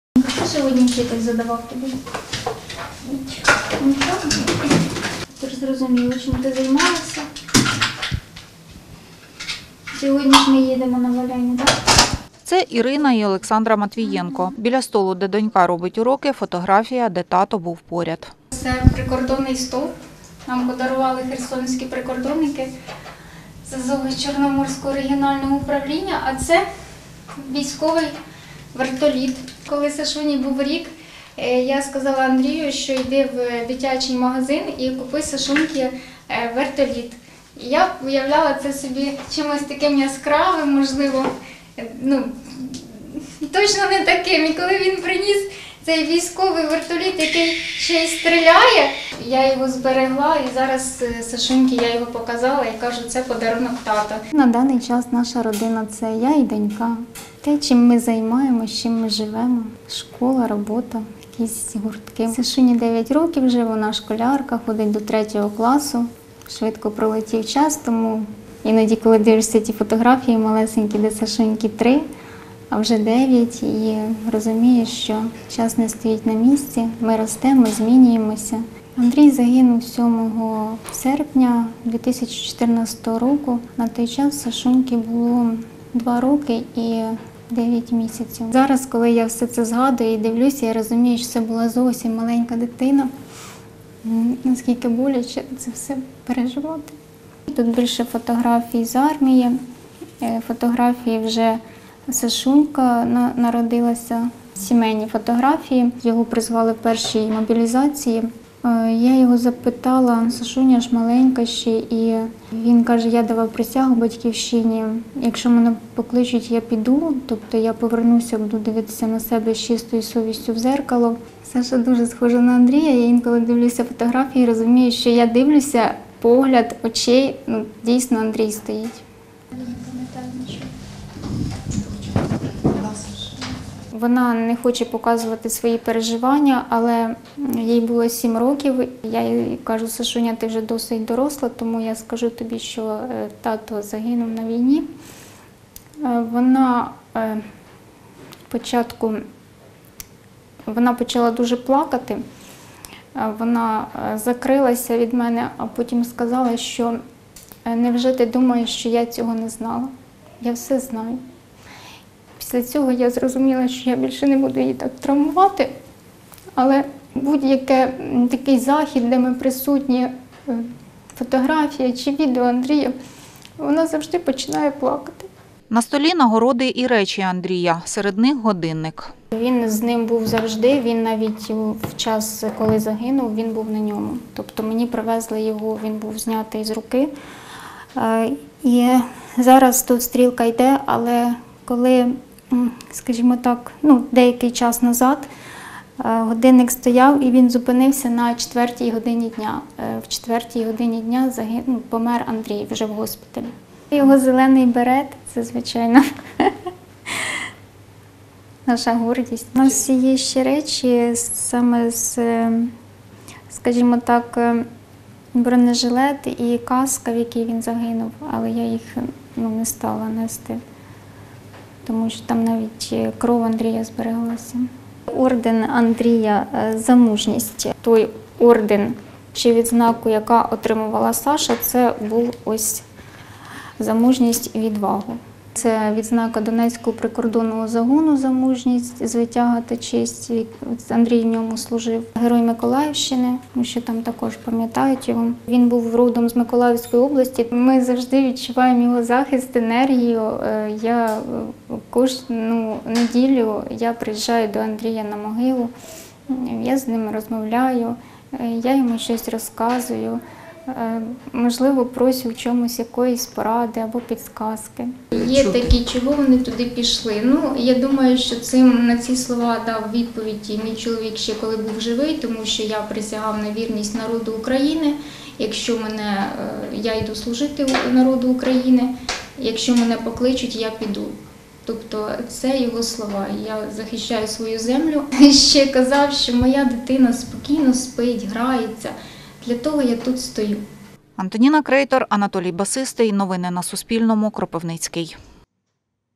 Орден “За мужність” Андрія Матвієнка отримувала його донька
Андрій Матвієнко загинув у 2014 році поблизу селища Лисиче, на кордоні Донецької області. Він віз 30 поранених у шпиталь і підірвався на фугасній міні. У Кропивницькому вулицю Башкірська перейменували на вулицю Андрія Матвієнка.

Протягом тижня до 6 грудня – Дня Збройних сил України – Суспільне Кропивницький розповідає про військових Кіровоградської області, які служили або служать в українській армії. Це третя історія.